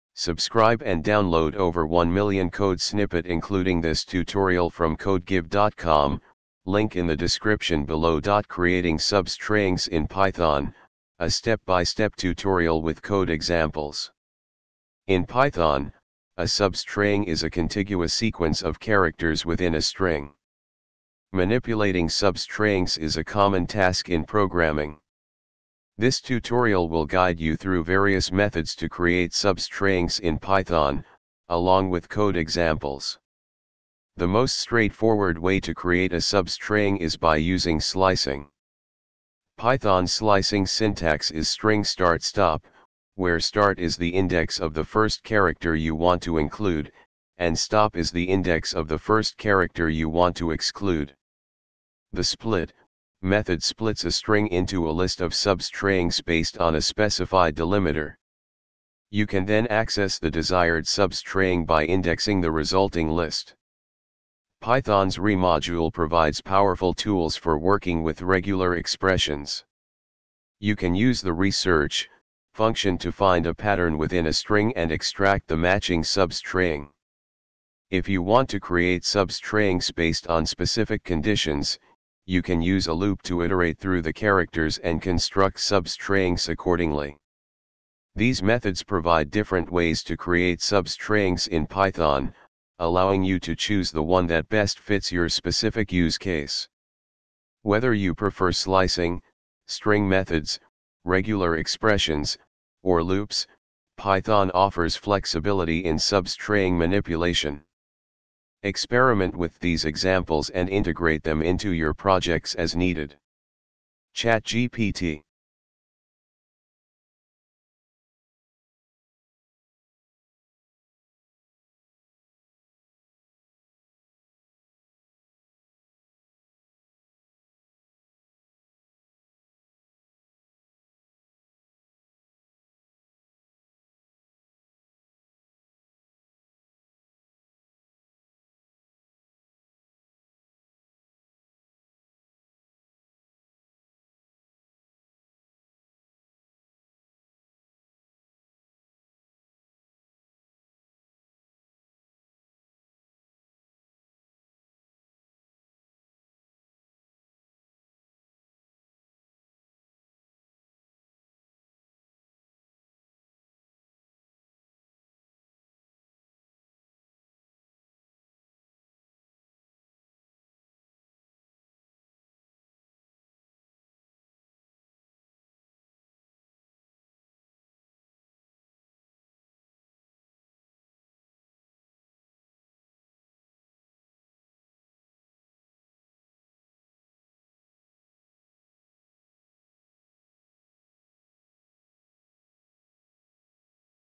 python create substring
Creating Substrings in Python: A Step-by-Step Tutorial with Code Examples
In Python, a substring is a contiguous sequence of characters within a string. Manipulating substrings is a common task in programming. This tutorial will guide you through various methods to create substrings in Python, along with code examples.
The most straightforward way to create a substring is by using slicing. Python's slicing syntax is string[start:stop], where start is the index of the first character you want to include, and stop is the index of the first character you want to exclude.
The split() method splits a string into a list of substrings based on a specified delimiter. You can then access the desired substring by indexing the resulting list.
Python's re module provides powerful tools for working with regular expressions. You can use the re.search() function to find a pattern within a string and extract the matching substring.
If you want to create substrings based on specific conditions, you can use a loop to iterate through the characters and construct substrings accordingly.
These methods provide different ways to create substrings in Python, allowing you to choose the one that best fits your specific use case. Whether you prefer slicing, string methods, regular expressions, or loops, Python offers flexibility in substring manipulation. Experiment with these examples and integrate them into your projects as needed.
ChatGPT